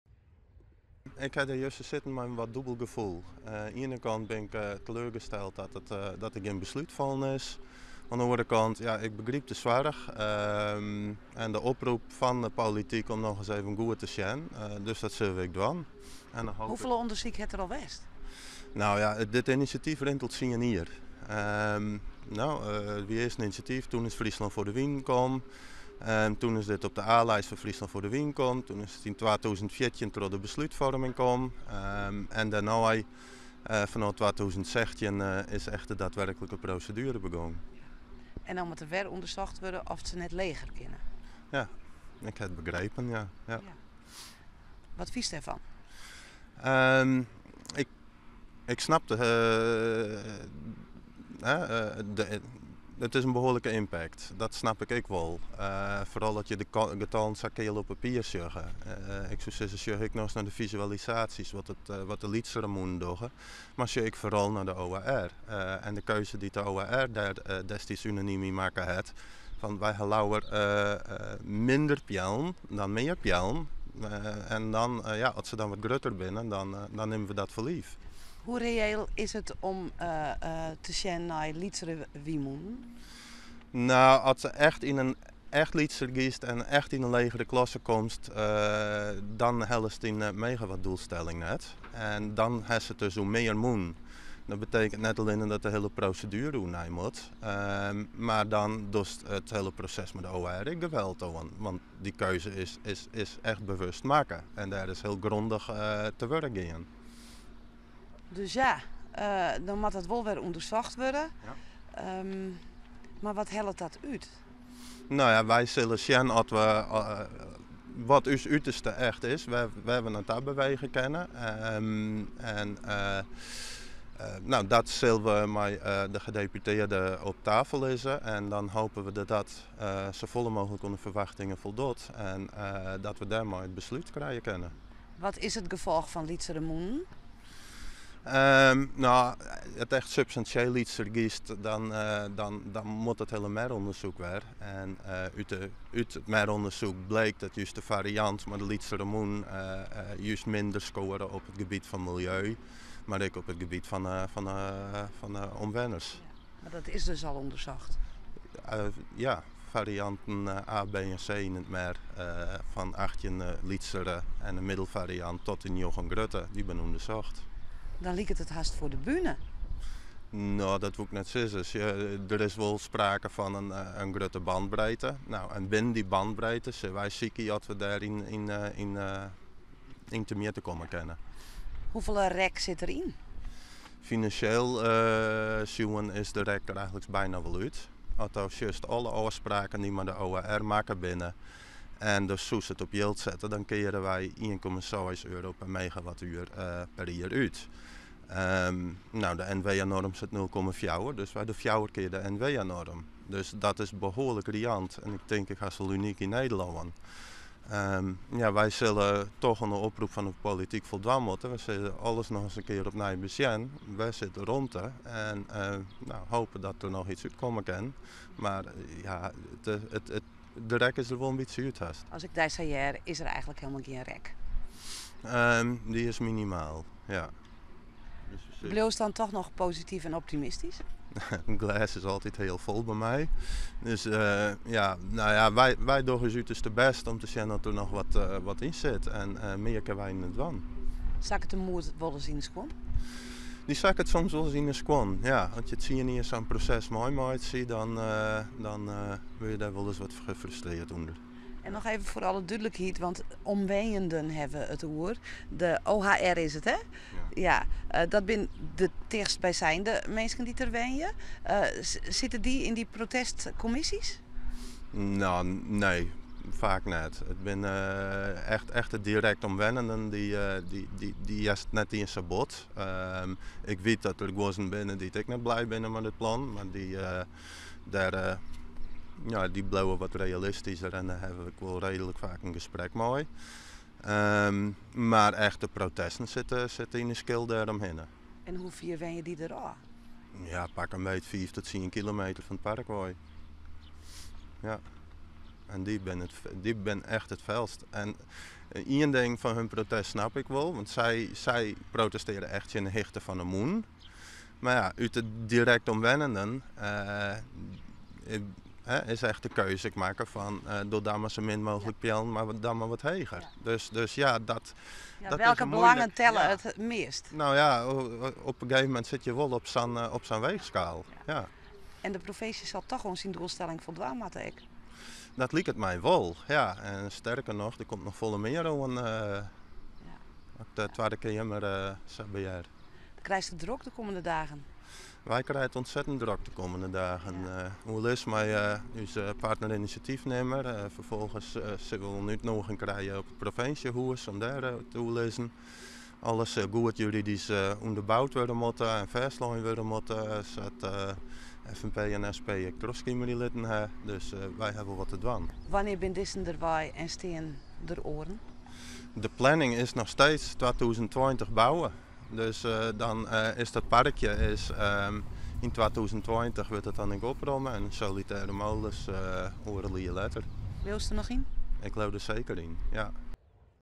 REPO: Teleurstelling en zorgen bij initiatiefnemers Nij Hiddem-Houw
Sjoerd Mendison, de man die het initiatief heeft genomen voor windmolenpark Nij Hiddum-Houw, is teleurgesteld met het uitstel van het park. Hij begrijpt dat er zorgen zijn, maar vindt ook dat er zorgvuldig met de omwoners is gesproken.